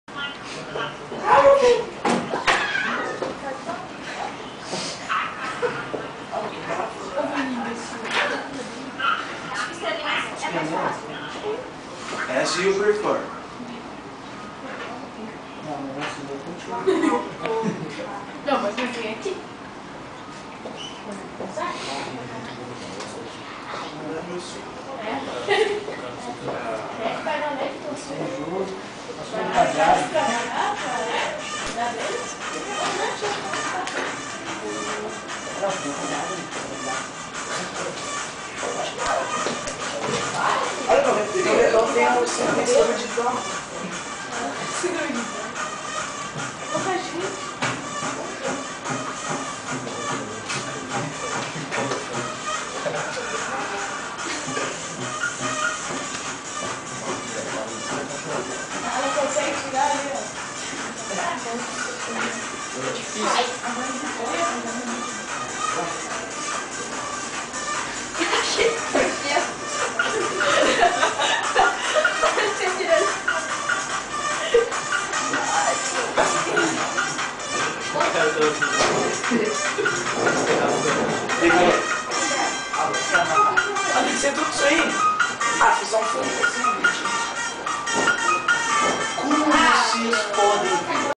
Halloween Workshop: Making your own Jack O'Lantern
This video was recorded at Senac 24h (Porto Alegre - Shopping Lindóia) while teacher Maicon (the vampire!) was teaching students how to make their own Jack O'Lantern.